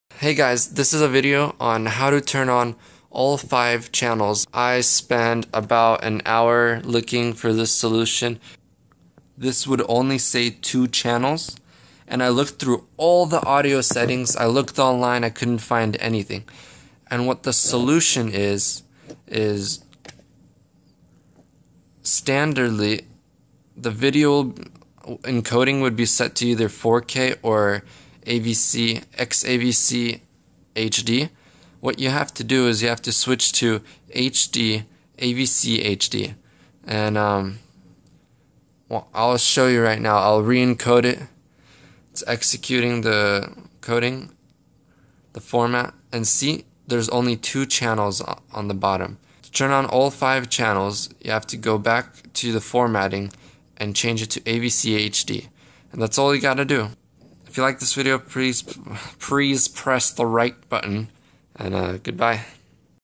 How to turn on 5.1 Channel on Sony FDR ax 53
My Camera
My Microphone
My editing software

After searching for an hour I discovered a simple solution for an annoying problem. How to turn on 5.1 channel audio for the sony ax53.